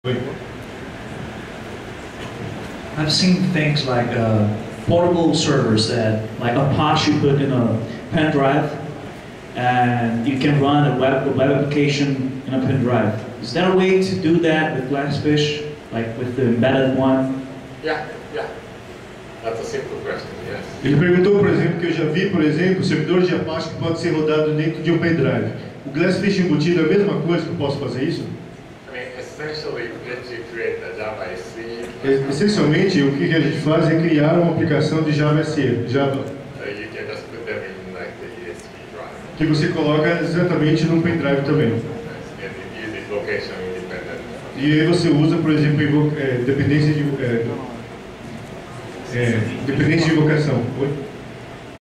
Cejug - Aniversário do Cejug 2008 - Kohsuke Kawaguchi - Perguntas 2
Kohsuke Kawaguchi, engenheiro senior da Sun Microsystems e possuidor de inúmeros projetos de dentro do Java.net, respondendo a perguntas após ter apresentado uma palestra no Aniversário de 6 anos do Cejug, com o tema "Glassfish v3: A nova geração de Servidores de Aplicação modular", acontecida na faculdade FA7 no dia 16 de Setembro de 2008 em Fortaleza, Ceará, Brasil."